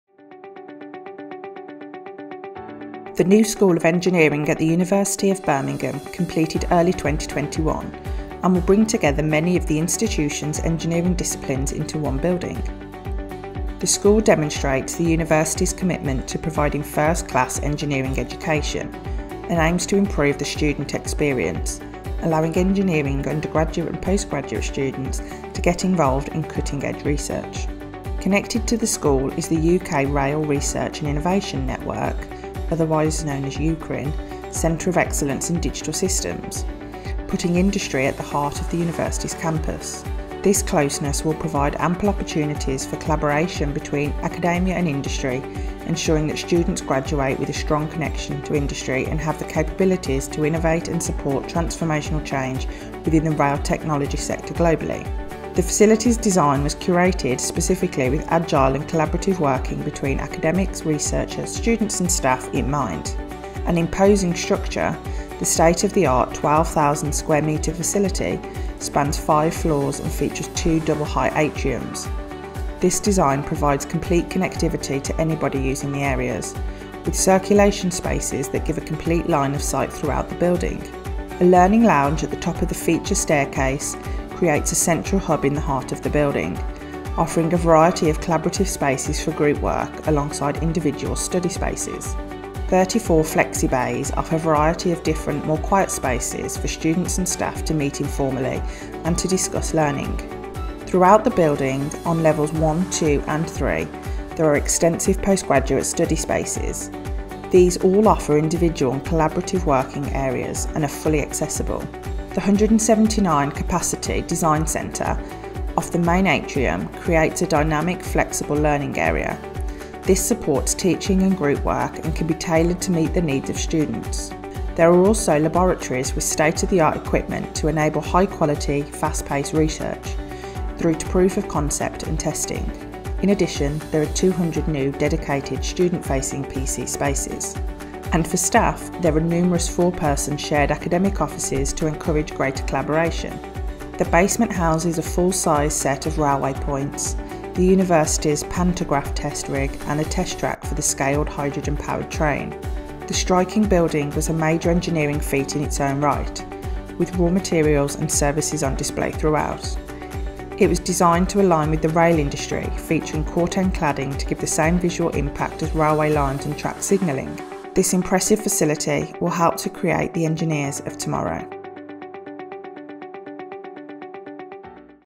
School of Engineering at the University of Birmingham
The new School of Engineering is part of the transformational redevelopment of the University of Birmingham, and our sixth project working together in four years.  The building has been designed with open plan spaces and cellular facilities to encourage agile and collaborative working between academics, researchers, students and staff.

Social value was at the heart of the project, as well a construction engagement programme ran with year 8 pupils in the local community, we also facilitated 157 student visits to site, hosted 71 work experience weeks with local people and enabled 202 weeks of ‘on site’ apprenticeship training.